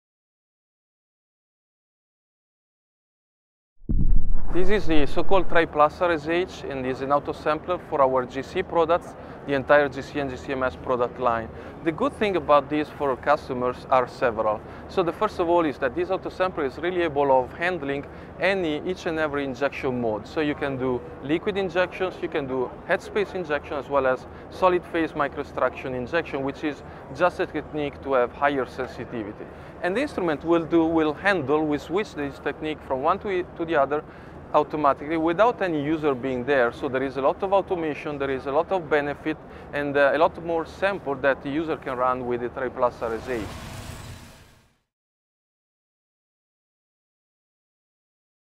Expert Insight - Massimo Santoro - Accelerate your GC - Thermo Scientific Autosampler
Excerpt of Massimo Santoro interview talking about the new Thermo Scientific TriPlus RSH autosampler accelerating GC and GC/MS Analysis in your lab.